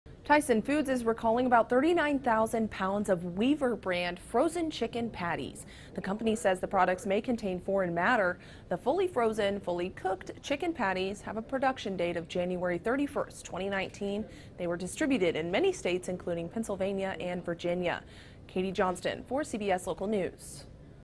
Tyson Foods Recalling 39K Pounds Of Weaver Brand Chicken Patties
Tysons Foods Inc is recalling around 39,000 pounds of Weaver brand frozen chicken patty product that may be possibly contaminated by foreign matter. Katie Johnston reports